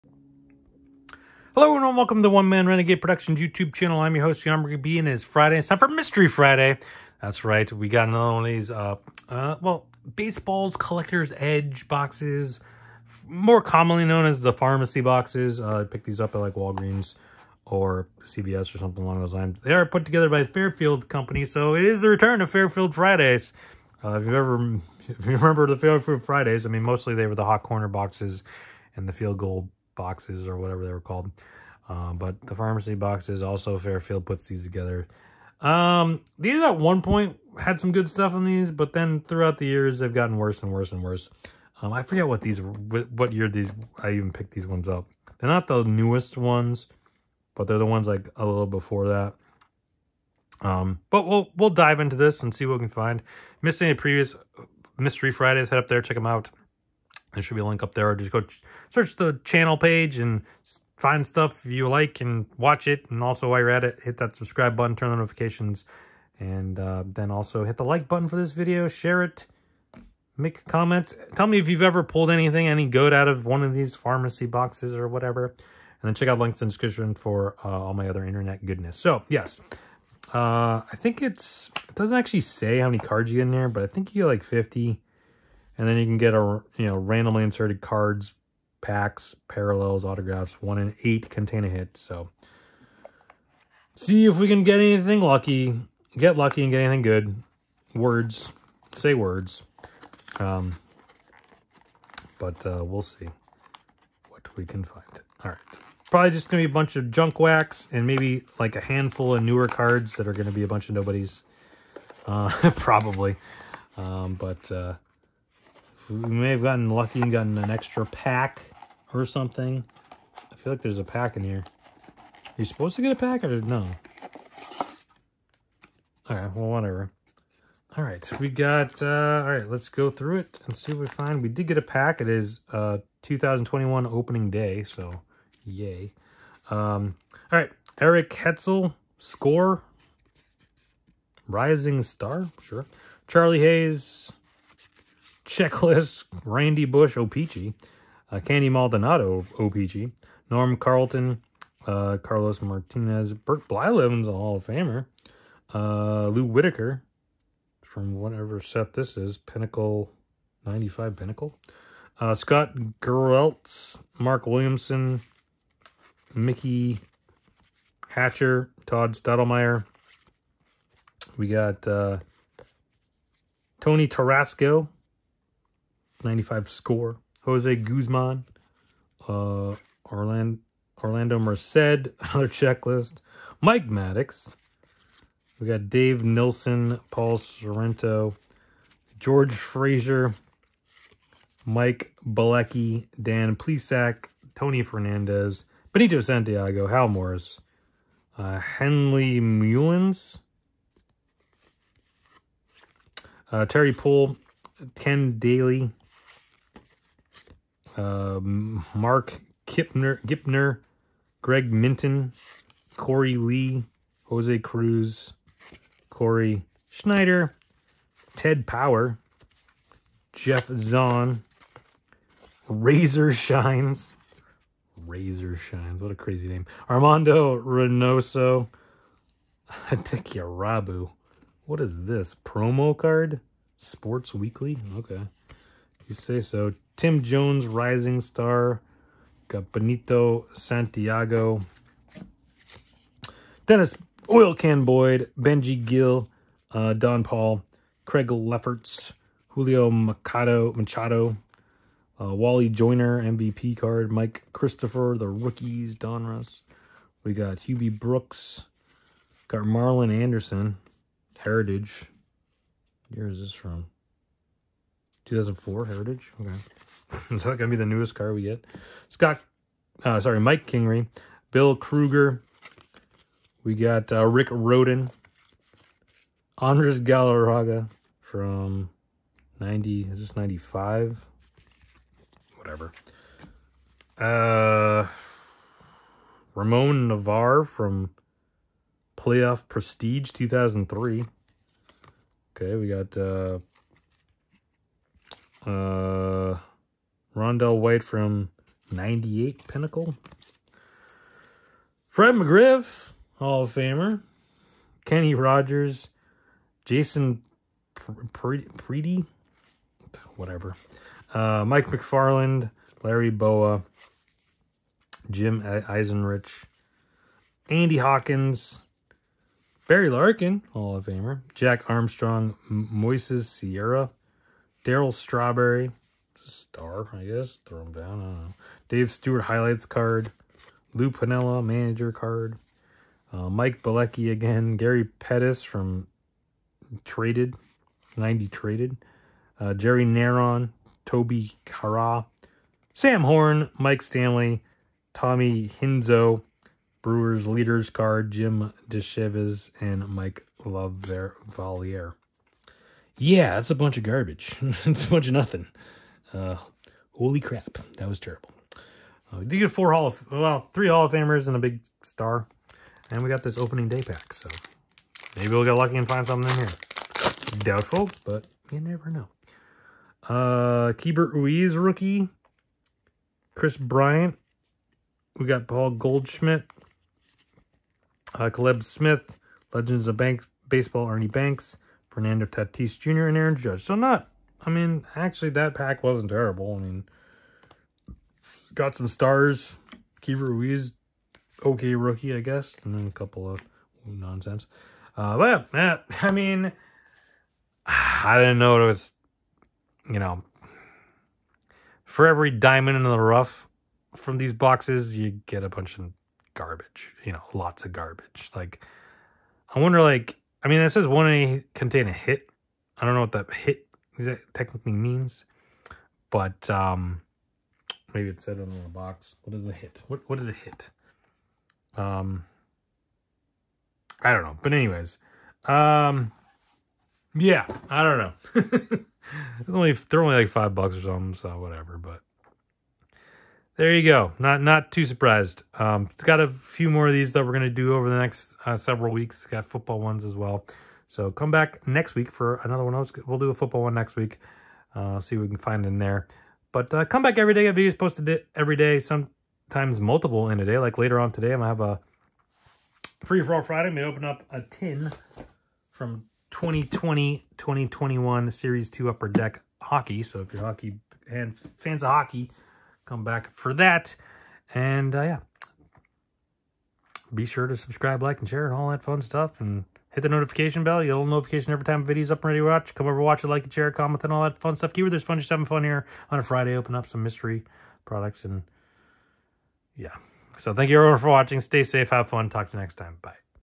Fairfield Friday: MLB Baseball Pharmacy Box #2! ⚾
It's Friday, you know what that means! Time again for Fairfield Friday! This time around I'm opening a MLB Baseball Pharmacy Box, will it all be junk, or is ther a diamond in the rough? Let's see how I do!

My Info:
Twitter or X or Whatever: @OneManRenegade